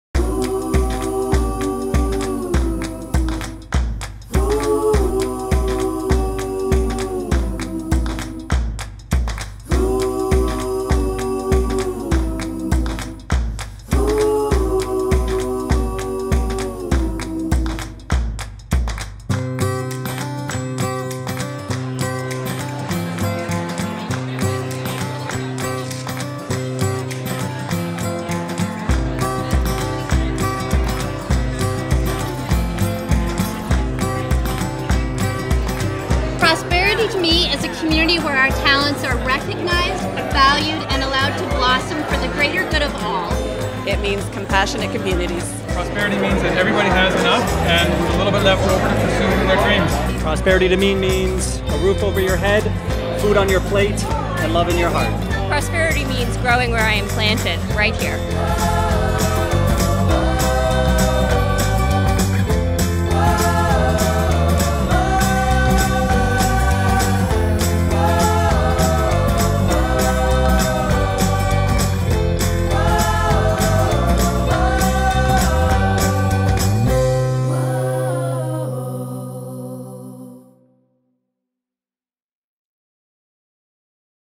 The Prosperity Project - Launch Event - Video 1
On April 9th, 2015, at events held across southwestern Ontario The Prosperity Project was unveiled as Libro’s innovative method to mobilize and inspire the people, businesses and communities of southwestern Ontario to grow prosperity together.